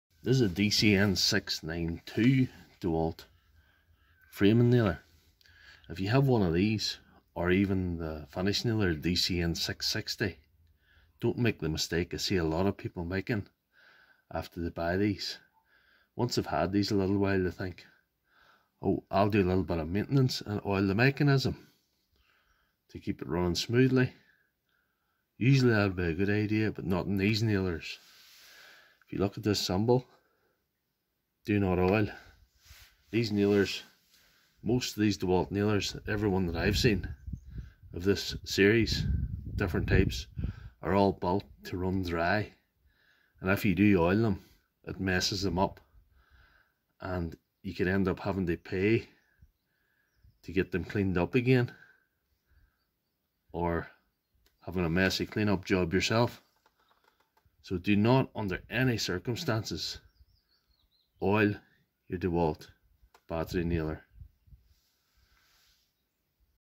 Got a DCN692? DON'T DO THIS!!
DCN692 Nail gun owners are making an extremely expensive mistake by overlooking a small sticker on the magazine. Dewalt nailers are designed to run dry and by adding any kind of lubricant or oil to the firing pin or any of the internal mechanical parts you are messing it up The same applies to the DCN660 and DCN690. What can you oil a dewalt nailer framer or finishing gun with? Absolutely nothing.
This is a common mistake